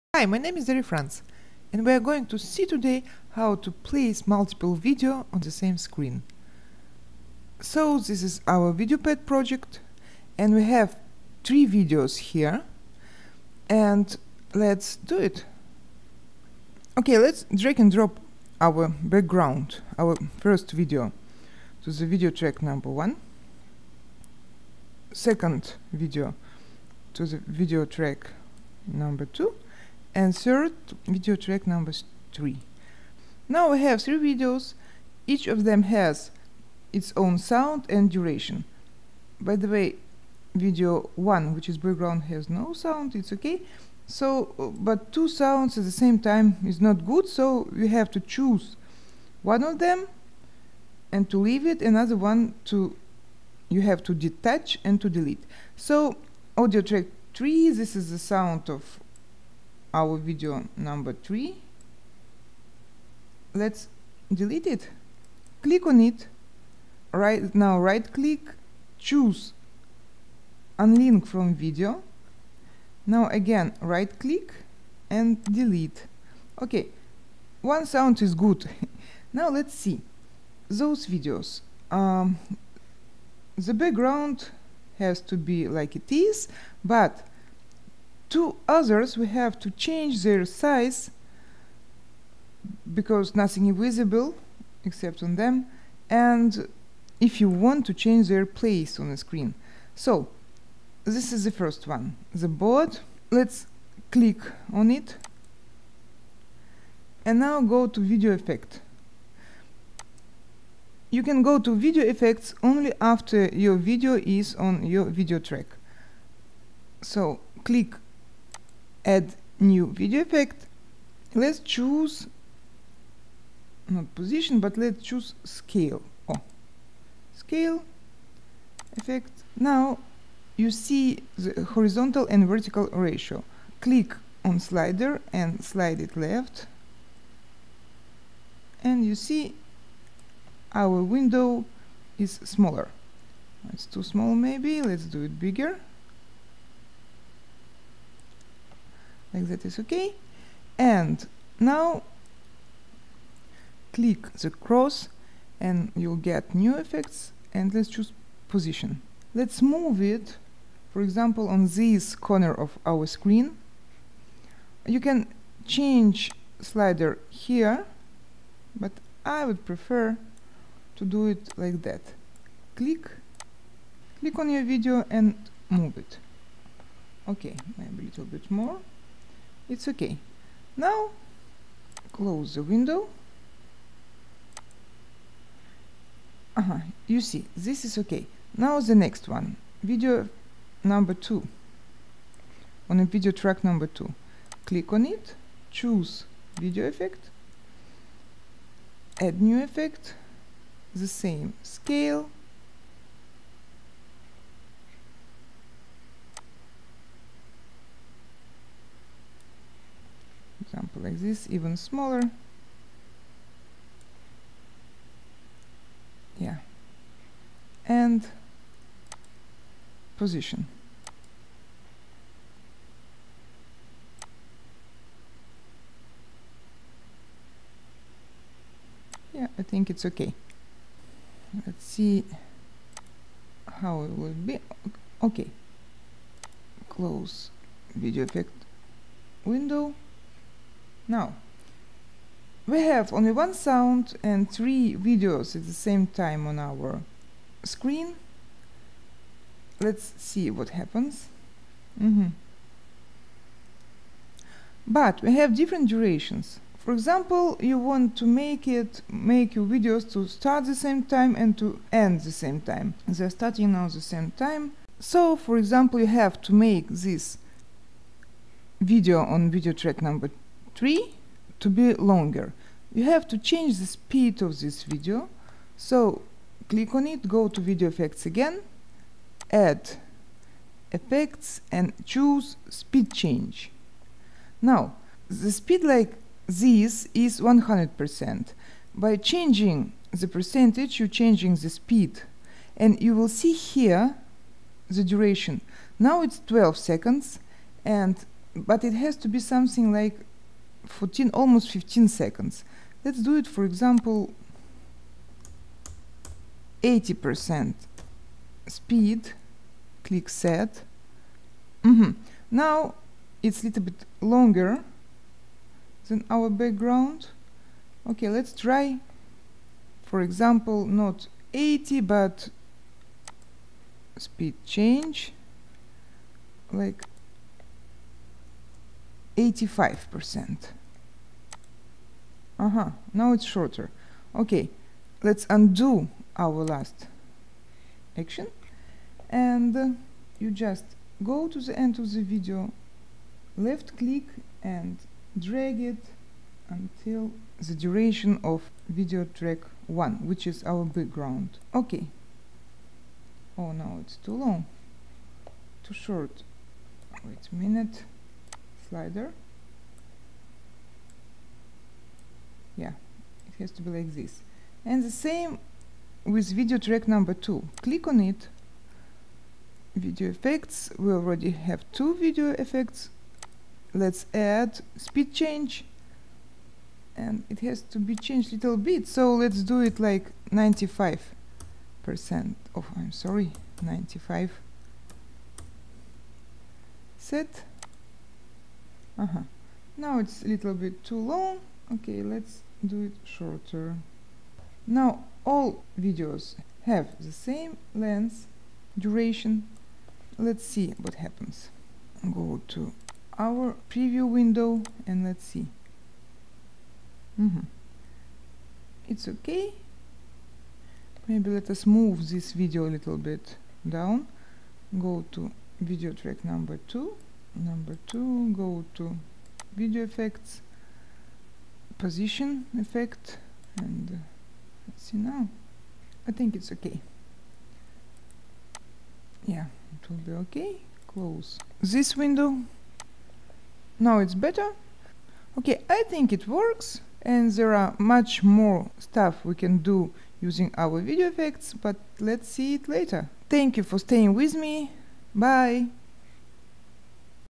How To Place Multiple Videos On The Same Screen In VideoPad |Video Effects | VideoPad Tutorials
Multiple Videos on the same screen. To do it you have to use few video effects.